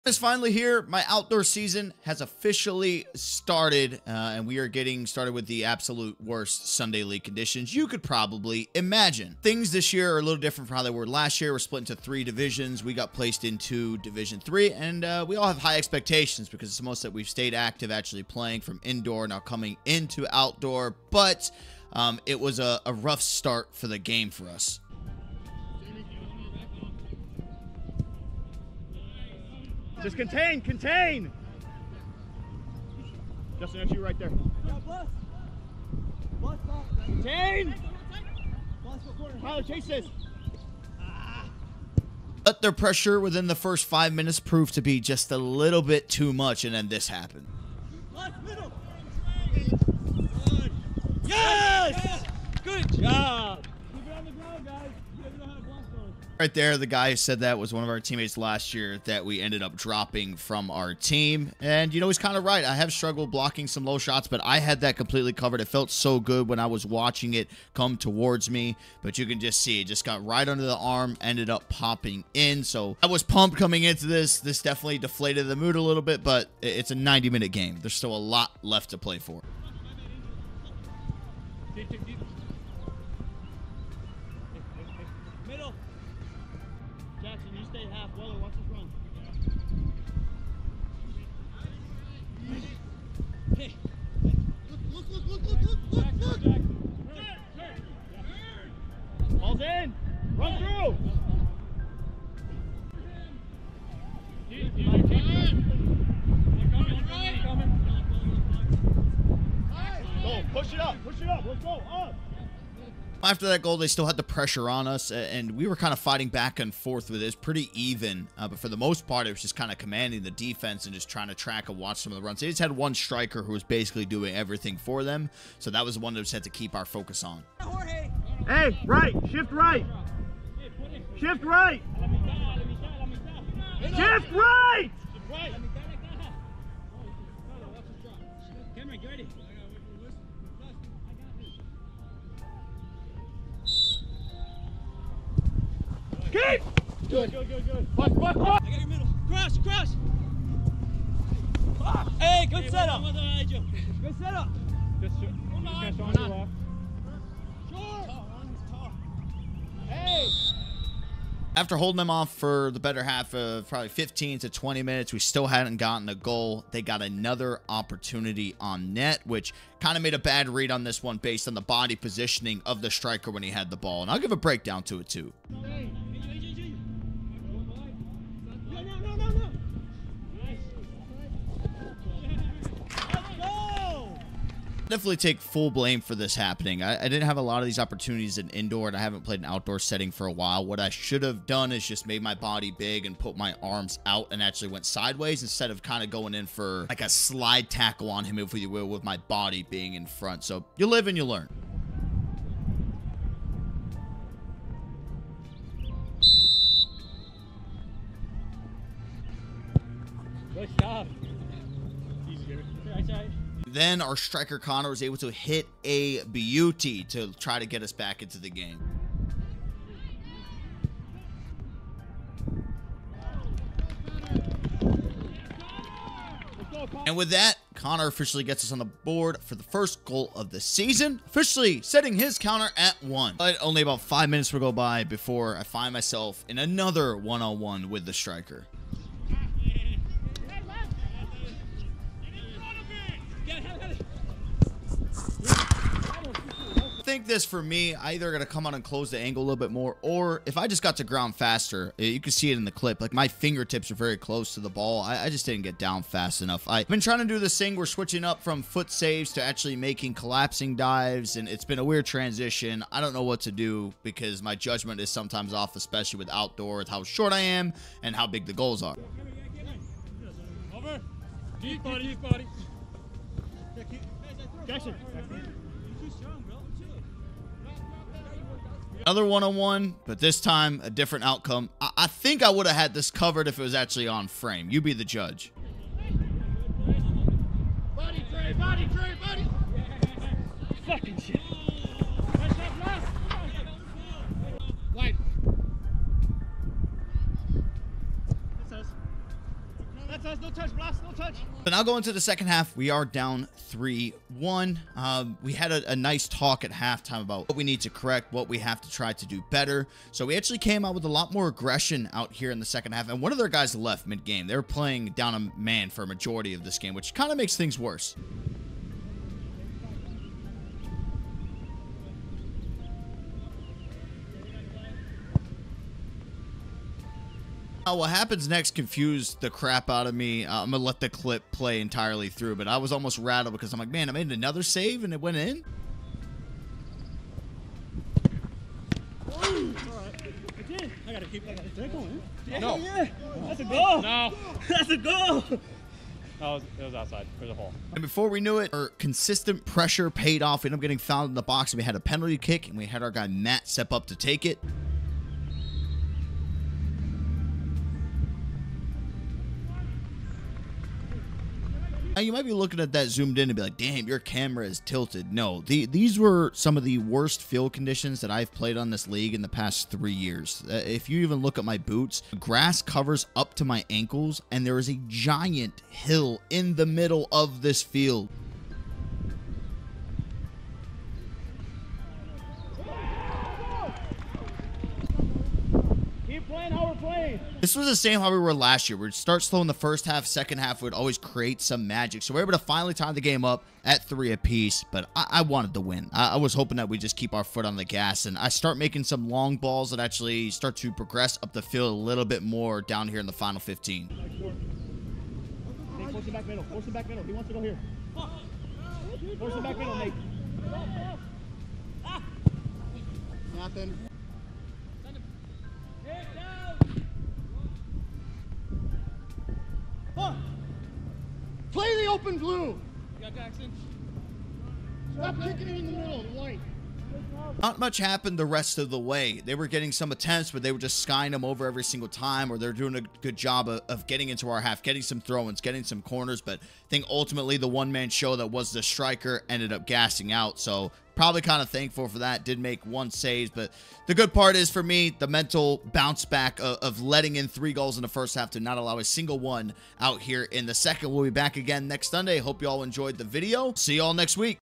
FIRST OUTDOOR GAME OF THE SEASON WAS A TIGHT ONE! - GOALKEPPER POV - ROCKFORD, IL
ALL SOCIALS CAN BE FOUND HERE!